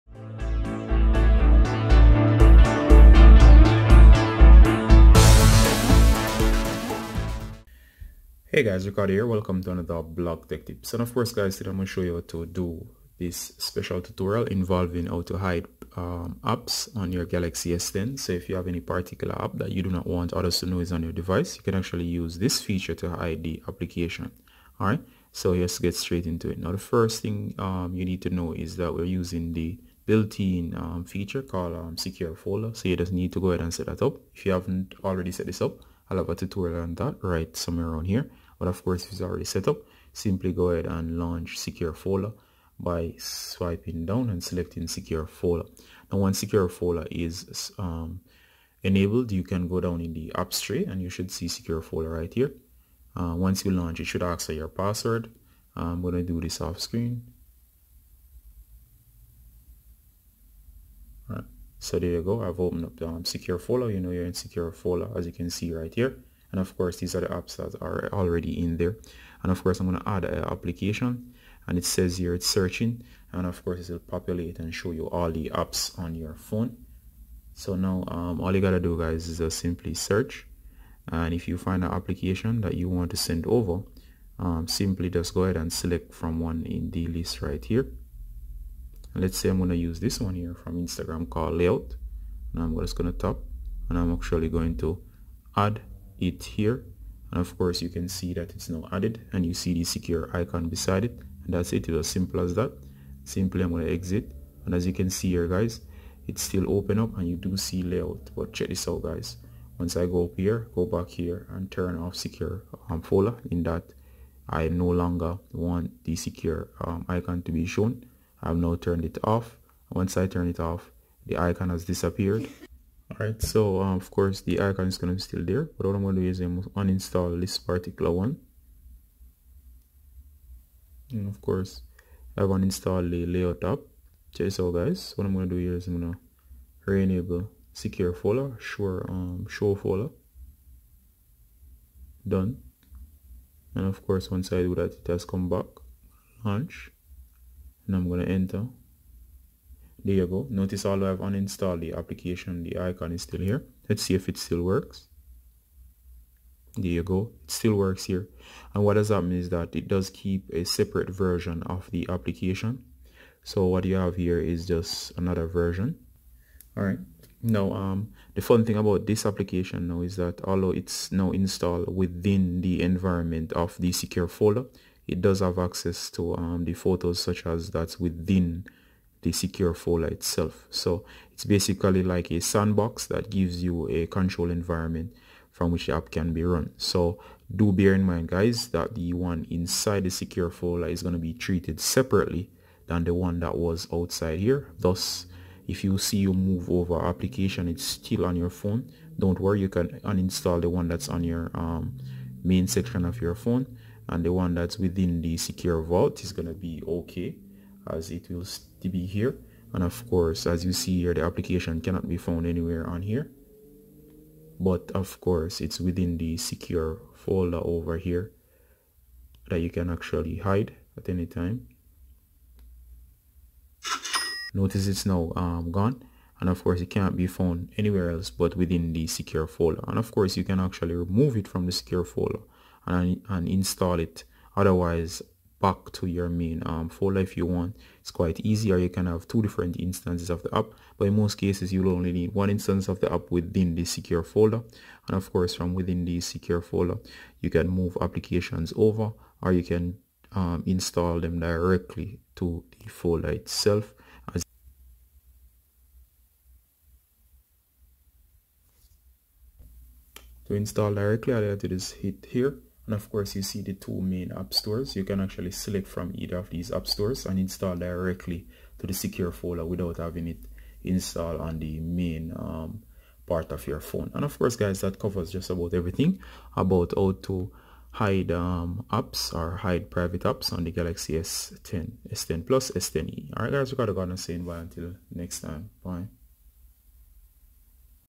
How to Hide Apps on the Galaxy S10 (Secure Folder)
This is how to hide apps Galaxy s10 tutorial. This also works on the S10e and S10 Plus as well. Hide your private app or sensitive ones you don't want others playing with or knowing about. This method uses the secure folder to both hide and password protect your Apps.